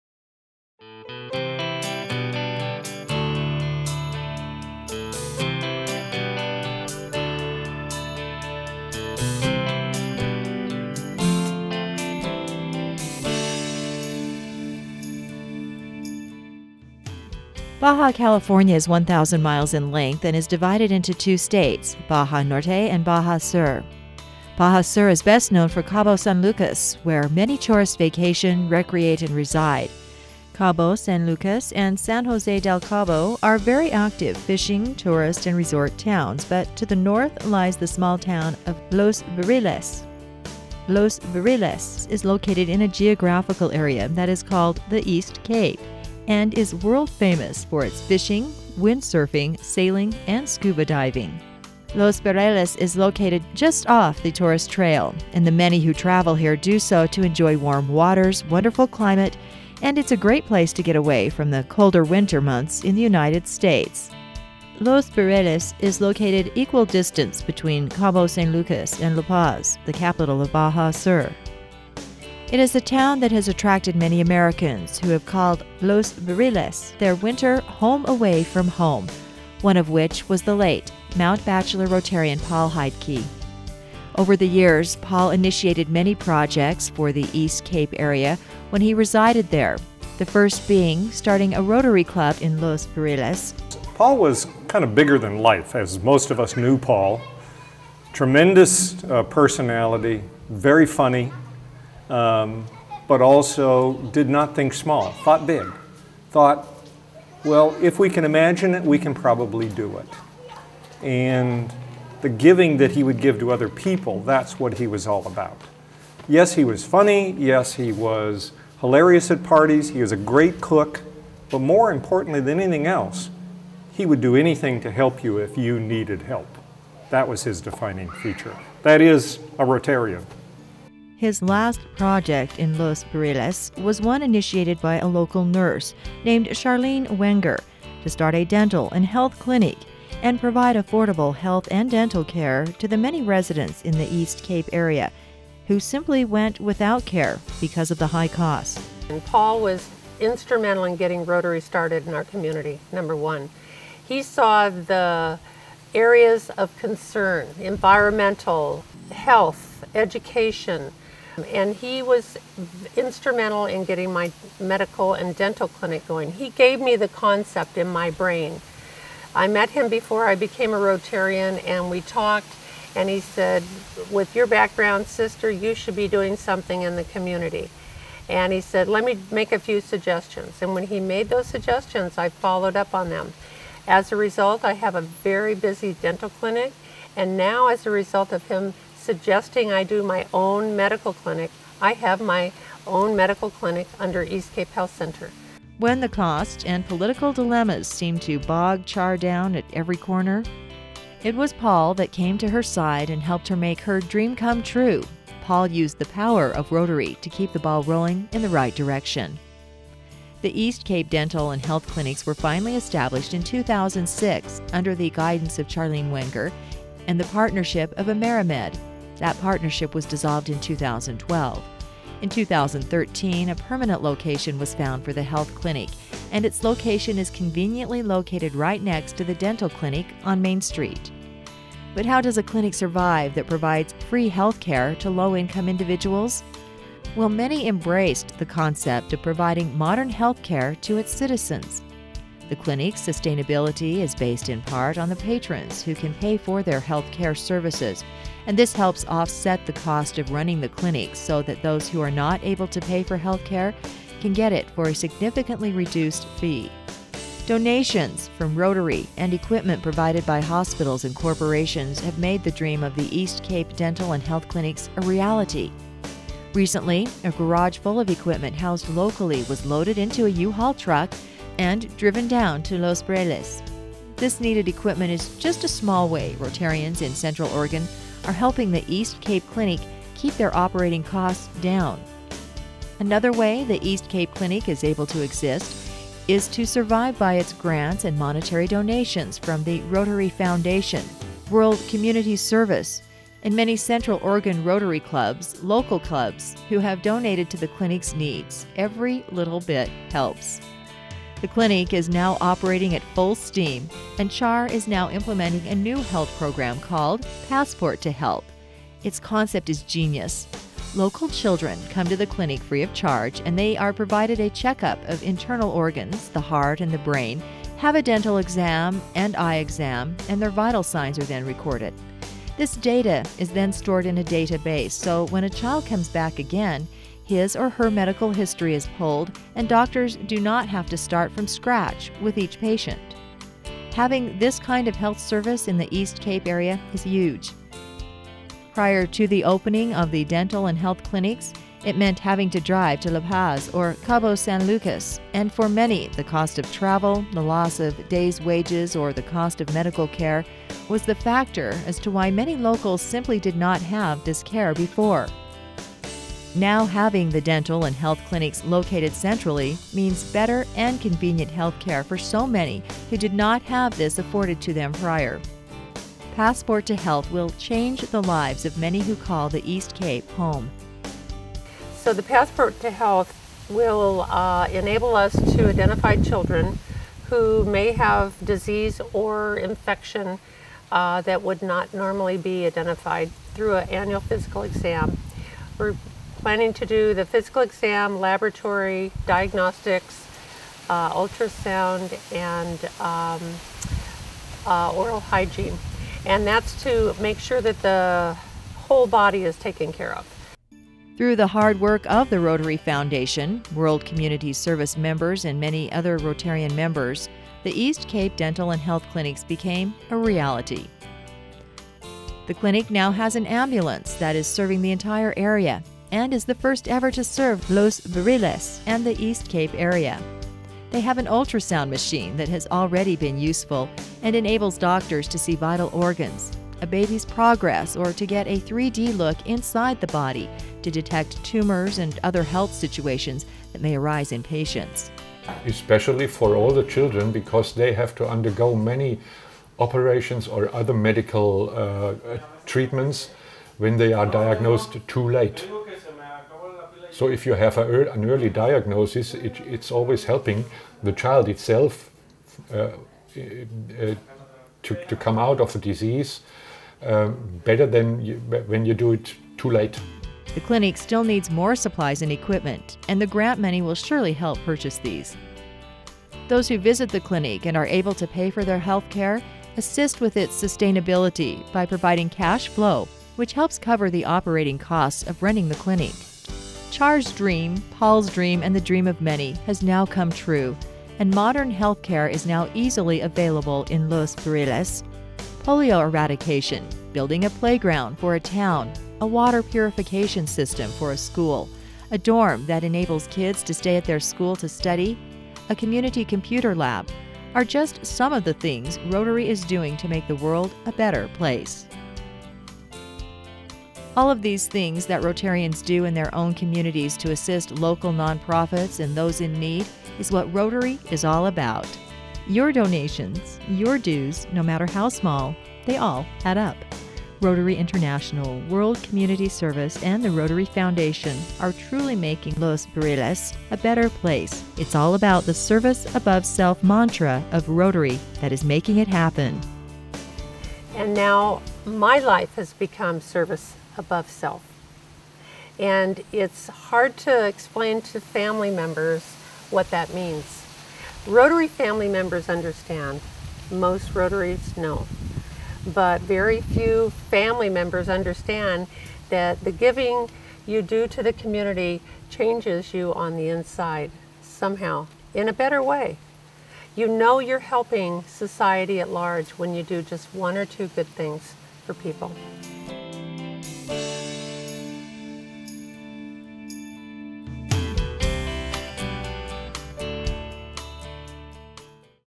Los Barriles East Cape Clinic mini doc 2014
This short mini doc is a tribute to Paul Heidtke, Mt. Bachelor Rotary, World Community Service, The Rotary Foundation and the many who helped establish the East Cape Health Clinics in Los Barriles, Baja Sur become a reality. It was the dream of Charlene Wenger, a Los Barriles nurse, whose vision to aid the East Cape residents need for free and cost effective health care came to be a reality through those whose ' Service Above Self" made the dental and health clinics a reality.